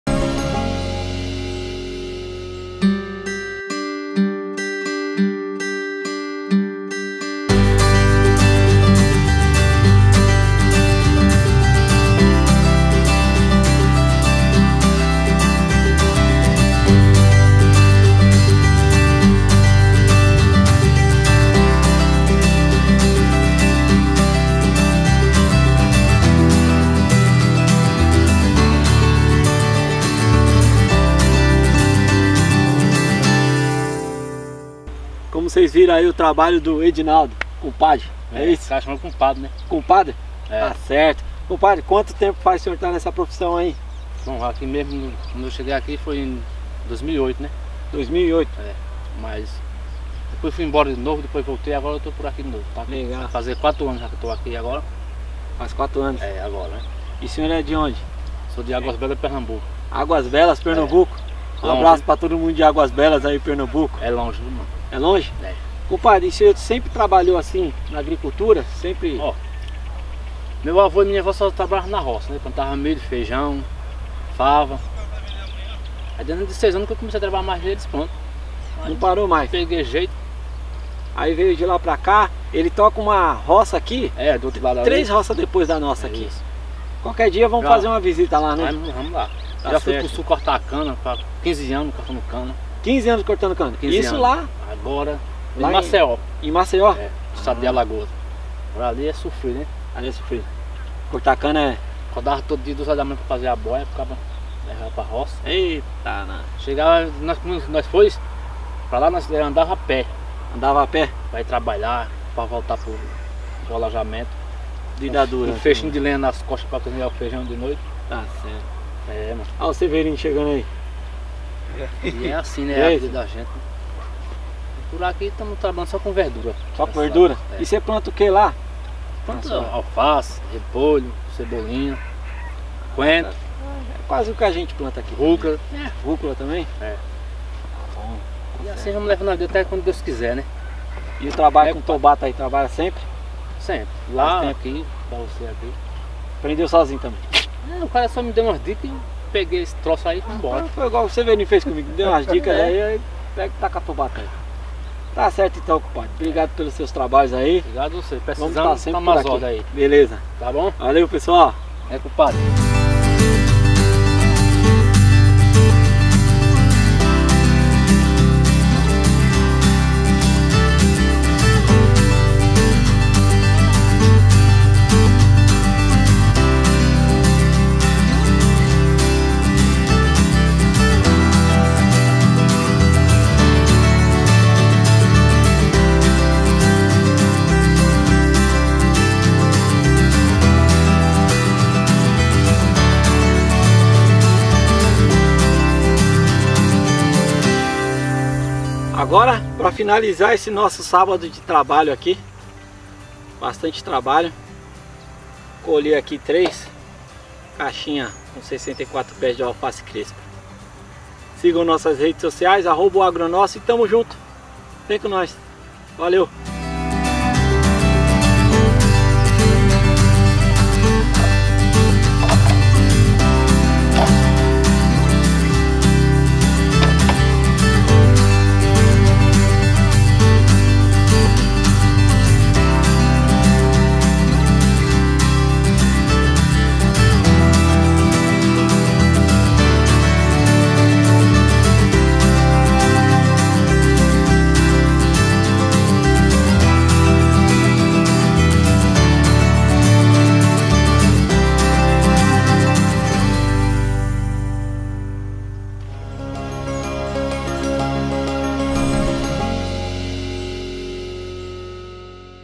COMPADRE DE PERNAMBUCO FAZENDO MINHA HORTA | AGRICULTURA FAMILIAR
Nesse sábado Marcos Meira teve a companhia do compadre para fazer os canteiros da horta do O Agro Nosso